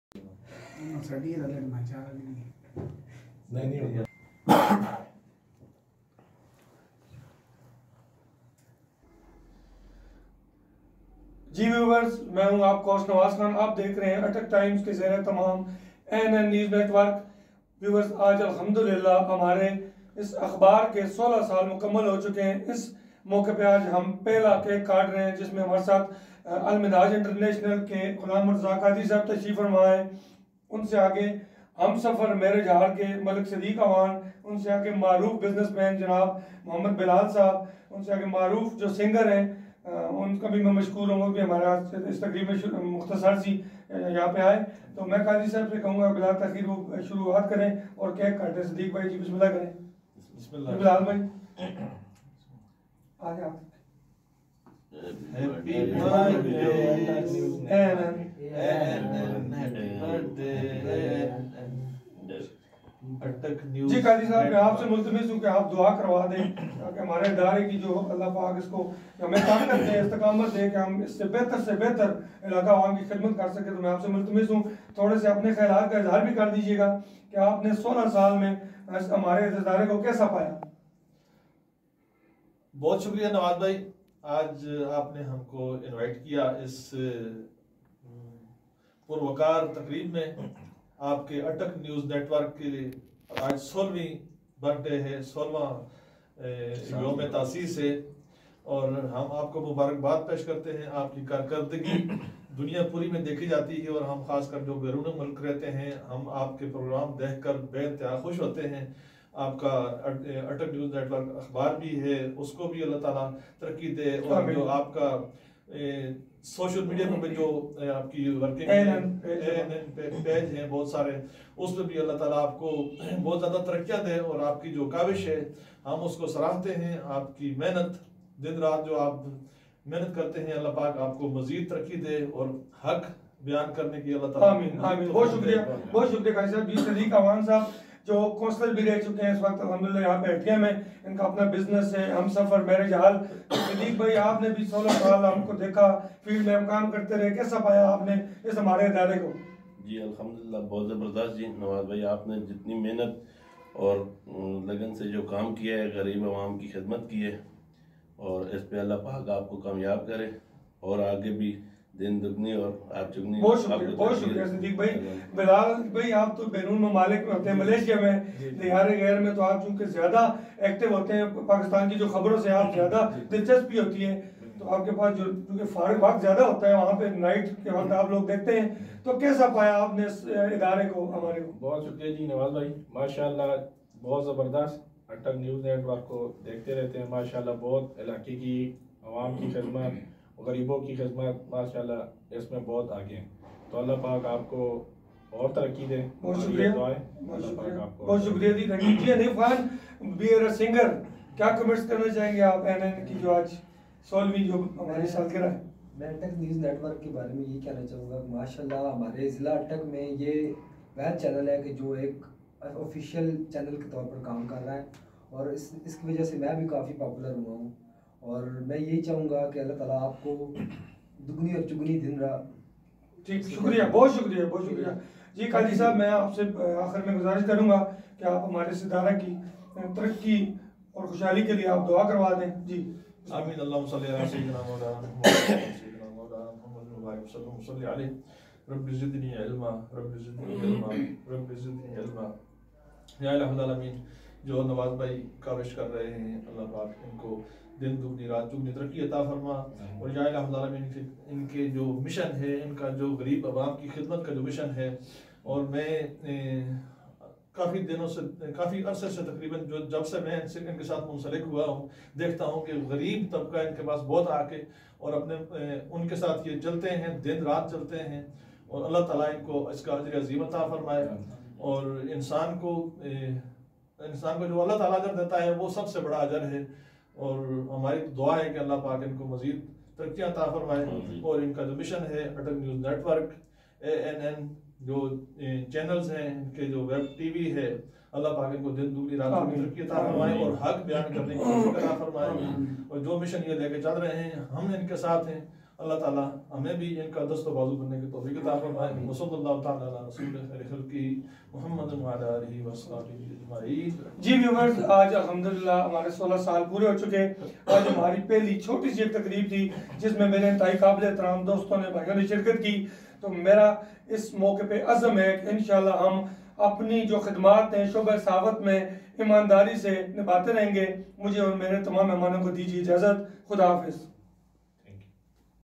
ANN اٹک ٹائمز واٹک نیوز نیٹ ورک کے16 سال
رب کائنات ادارہ اٹک نیوز نیٹ ورکANN کی پوری ٹیم کو سلامت رکھے اور دعا گو ہیں ادارہ سابقہ روایات کو برقرار رکھتے ہوئے شعبہ صحافت کے ذریعے عوامی خدمت کا مشن جاری رکھے ادارہ اٹک ٹائمز و ANNکی 16ویں سالگرہ کاکیک کاٹنے کی تقریب میں چیف ایڈیٹر اٹک نیوز نیٹ ورک نواز خان سے مہمانوں غلام مرتضی قادری ،چوہدری محمد بلال ،صدیق اعوان ،حنیف خان نے کیا گفتگو کی آپ بھی سنیں
Report by
Attock News Network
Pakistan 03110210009